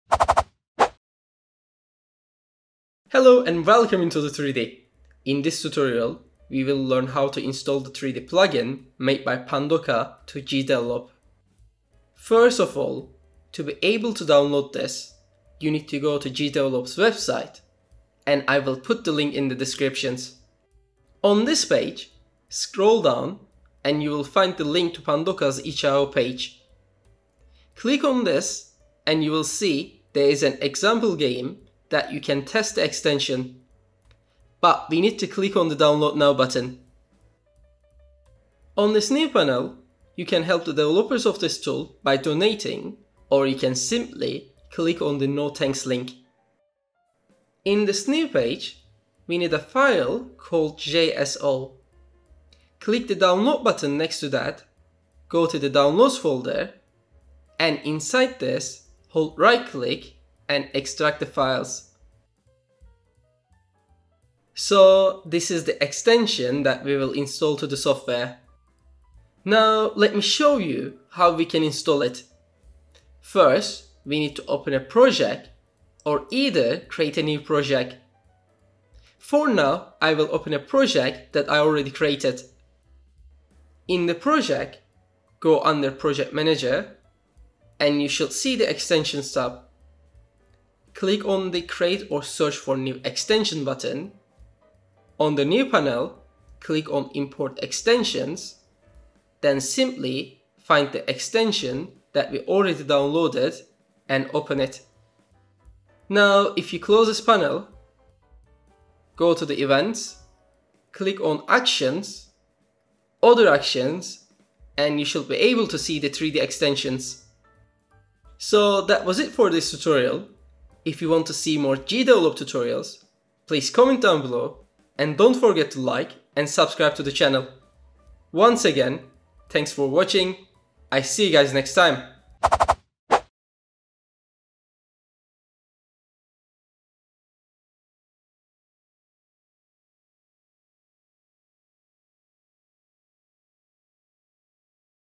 How to install "3D Extension" for GDEVELOP
Important:
1: Import extensions feature is only available for the desktop version of GDevelop! If you are using the website, It will most likely not be visible.
2: Pandako's 3D extension is now available inside GDevelop, All you need to do is, enable the show community extensions switch under the search for new extensions tab and search for WithThreeJSO!

- Key moments:
00:00 - Intro
00:16 - GDevelop Website
00:28 - Pandoka itch page
00:46 - Download the Extension
00:56 - Extract the files
01:14 - Open/Create Project
01:26 - Import the Extension
01:47 - Test
02:00 - Subscribe
02:11 - Outro